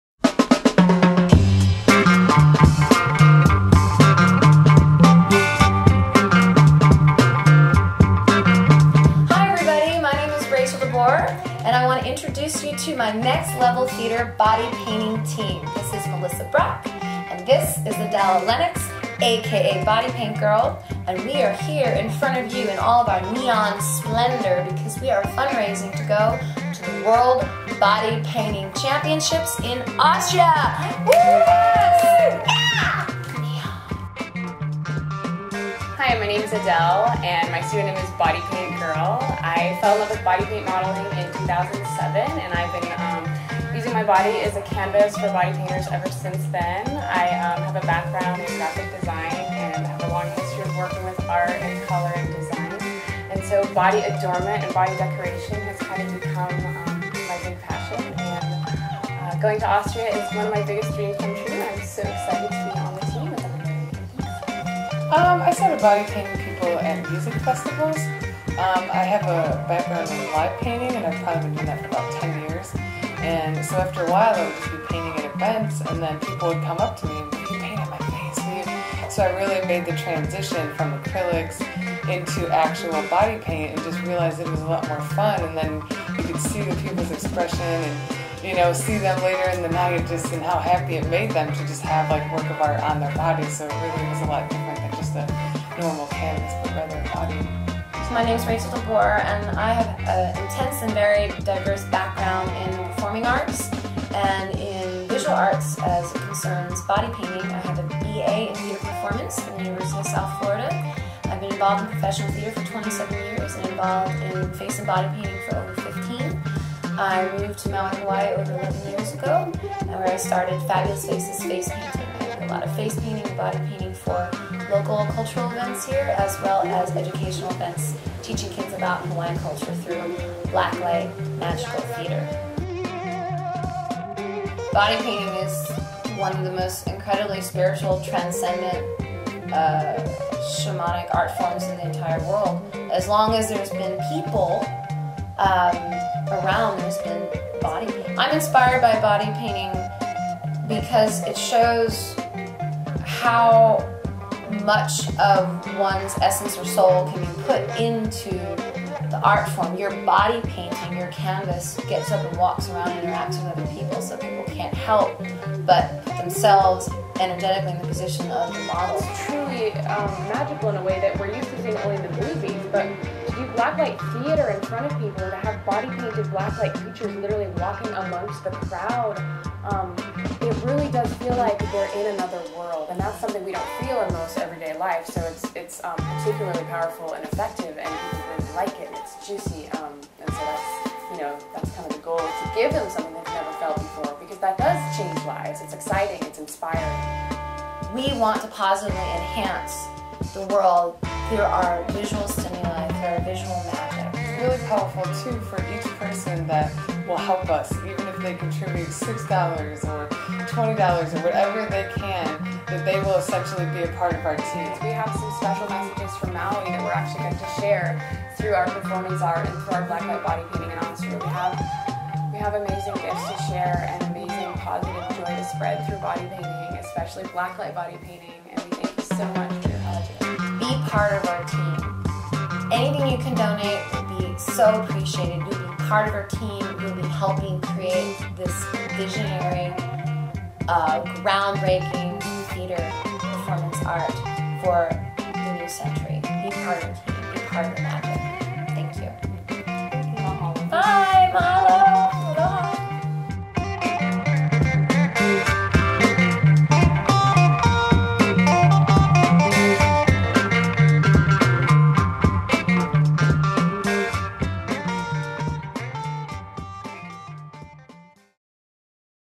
Help Next Level Theater go to the World Body Painting Festival in Austria
Our intrepid group of professional painters, models, and photographer have been accepted as one of only 15 competitors in the World Body Painting Festival's U.V. category... help us make our fundraising goal to get us to Austria to compete in this once in a lifetime opportunity!